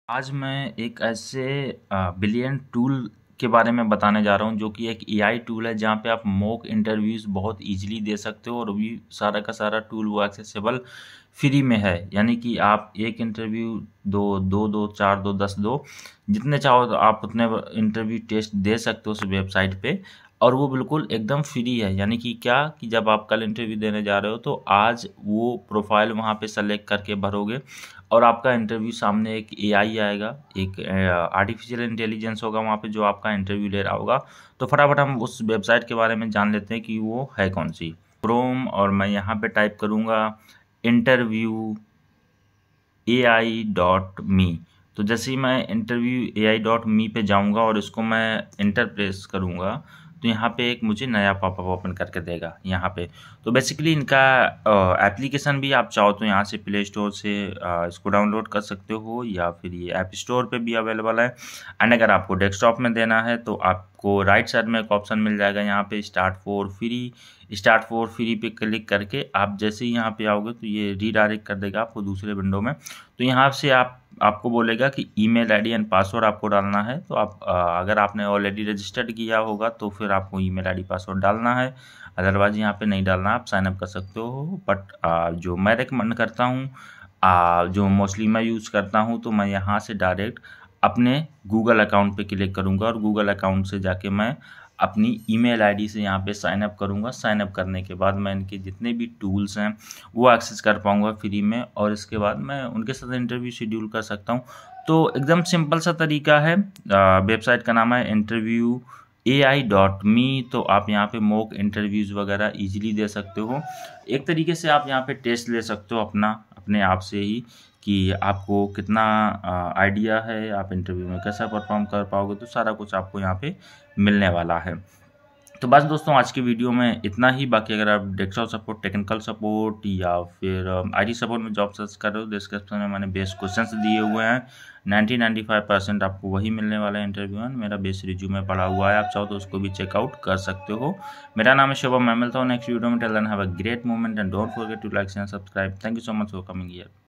Use This AI Tool for Mock interview in Free | Interview practice with AI 2024 |TCS & MNC Interview
Maine is video me btaya h ak ese tool ke bare me jha aap interview test kr sakte ho free me and apne aap dekh sakte ho ki aapka interview niklega ki nahi niklega full detail me hindi me discuss kiya i hope aapko video acha lage thanks...Use This AI Tool for Mock interview in Free | Interview practice with AI 2024 |TCS & MNC Interview

Thanks.